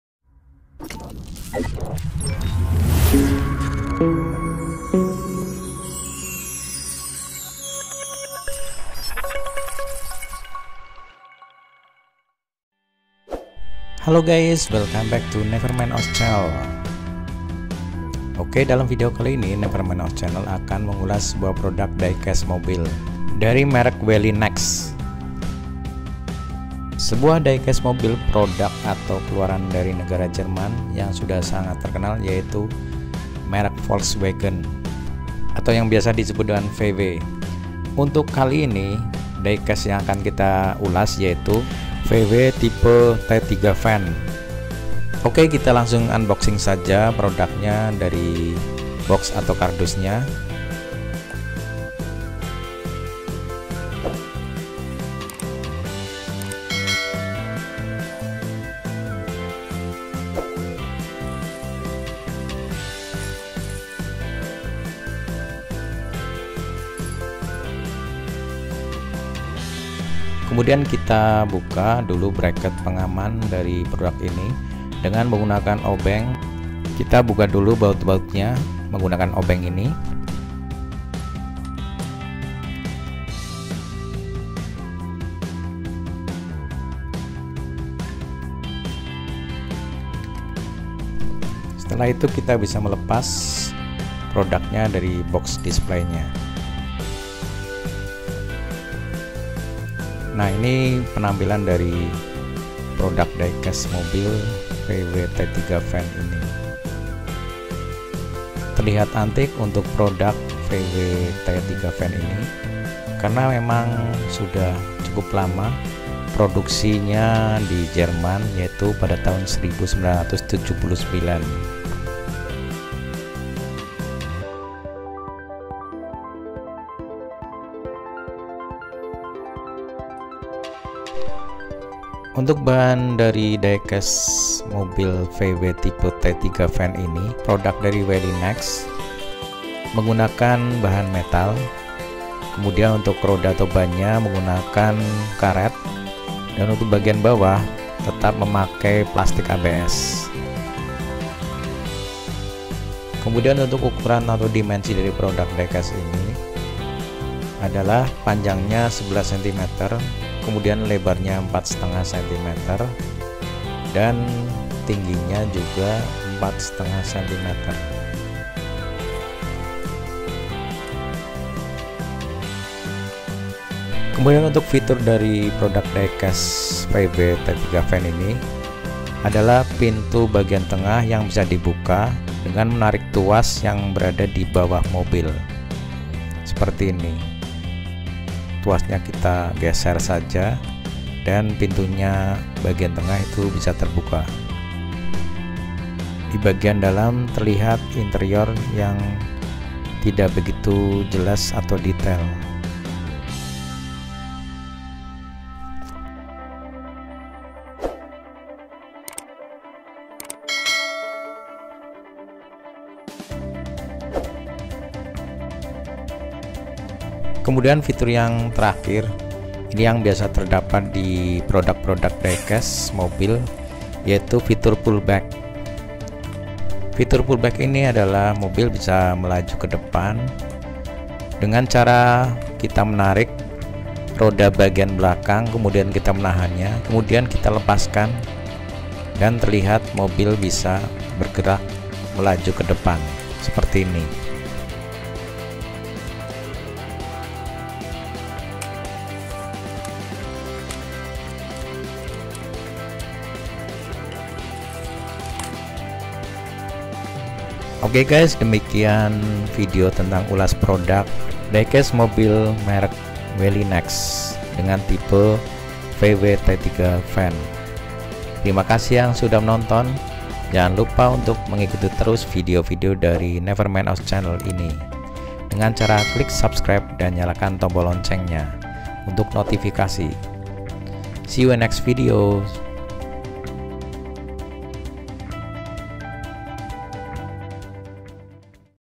ULAS PRODUK DIECAST MOBIL ANTIK VW T3 VAN MERK WELLY NEX SKALA 1:36
Ulas produk diecast mobil VW jenis T3 Van merk dari Welly Nex skala 1:36